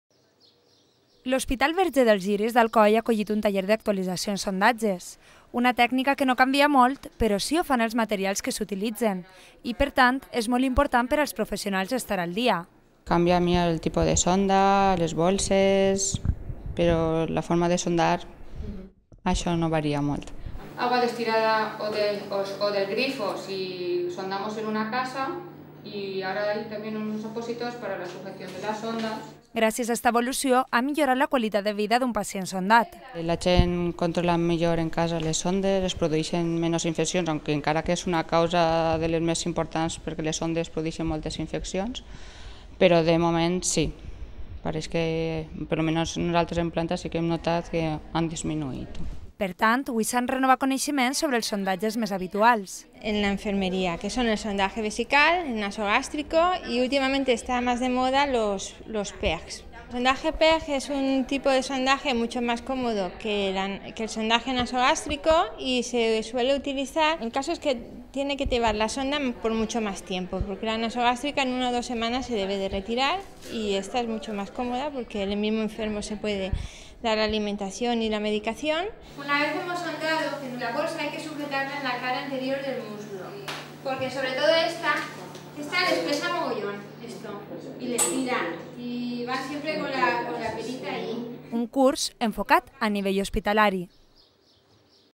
Taller de Actualización en Sondajes | Cecova TV
Los diferentes materiales que existen en el mercado y los pasos previos y posteriores a la colocación de una sonda. Han sido algunos de los conocimientos que han actualizado dos enfermeras de la Unidad Quirúrgica del Hospital de Alcoy a otras compañeras de profesión.